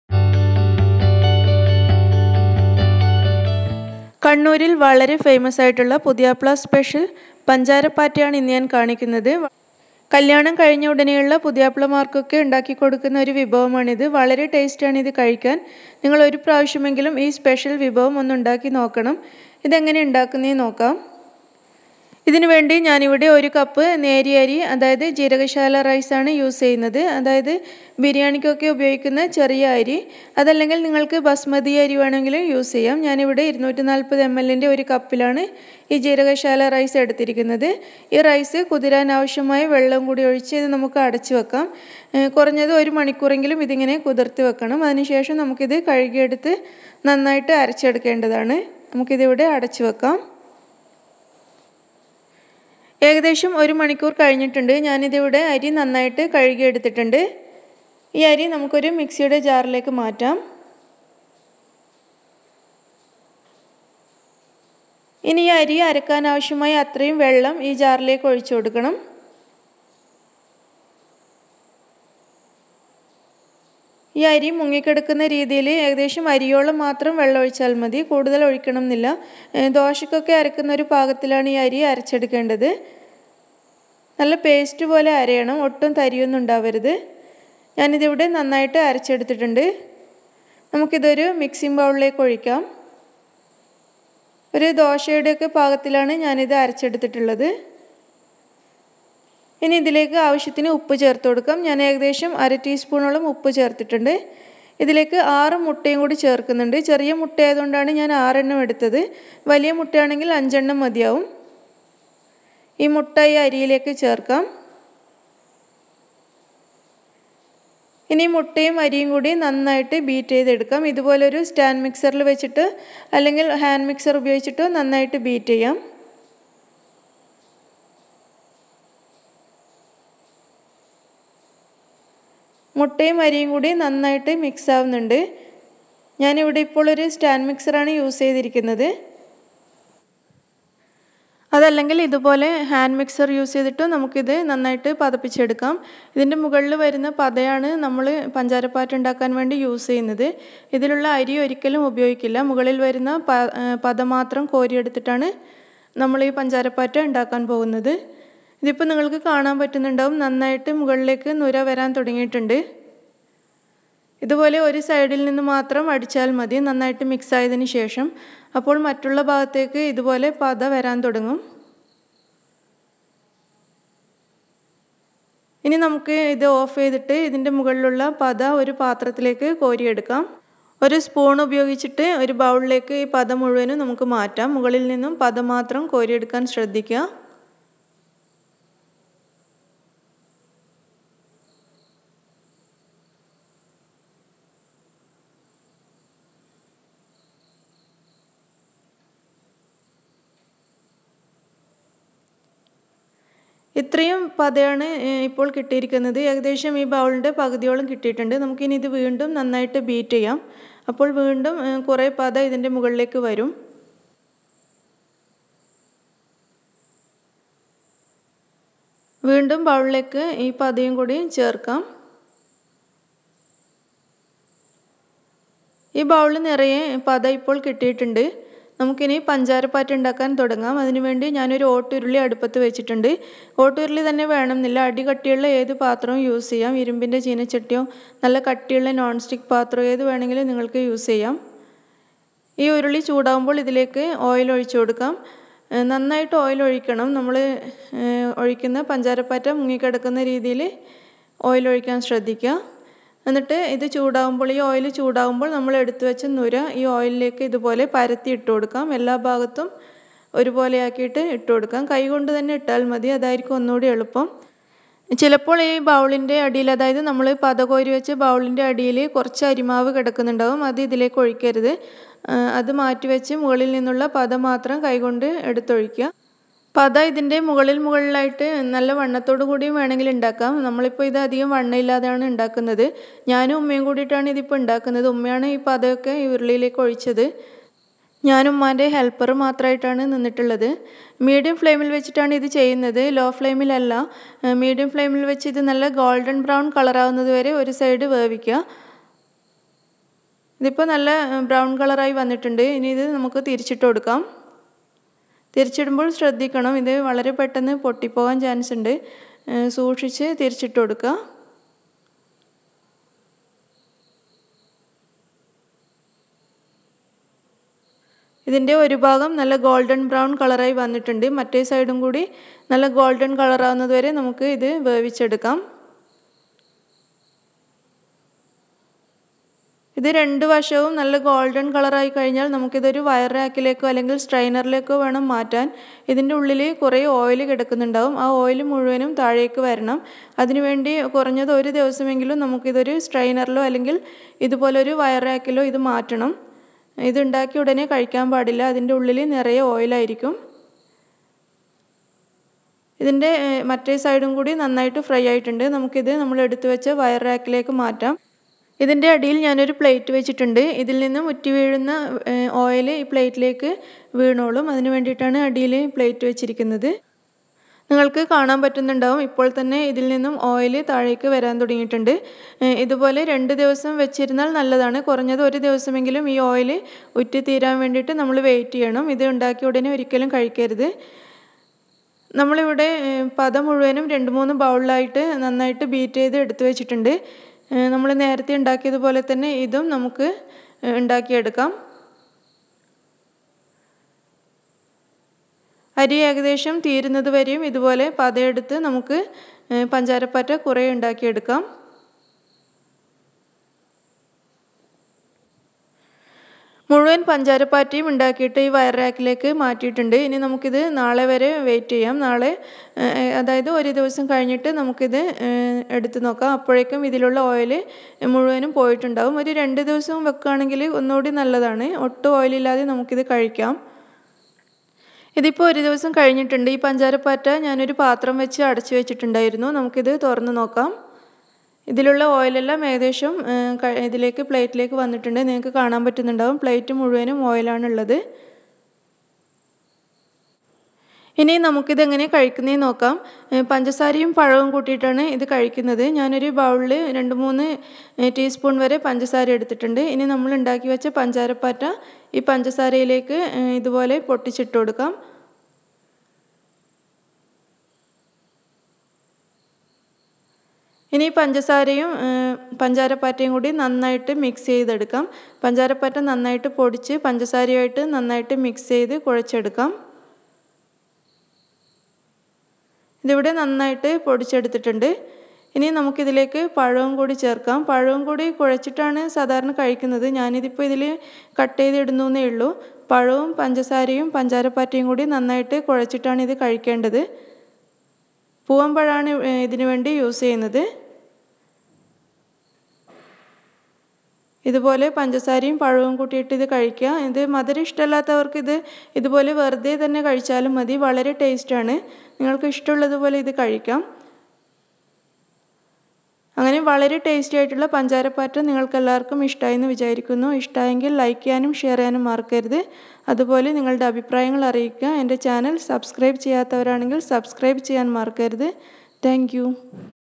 പഞ്ചാരപ്പാറ്റ //കണ്ണൂ൪ പുതിയാപ്ള സ്പെഷ്യൽ വിഭവം // Panchaara paatta
ingredients,
Jeerakasala rice -1 cup
eggs -6
salt
oil
sugar
banana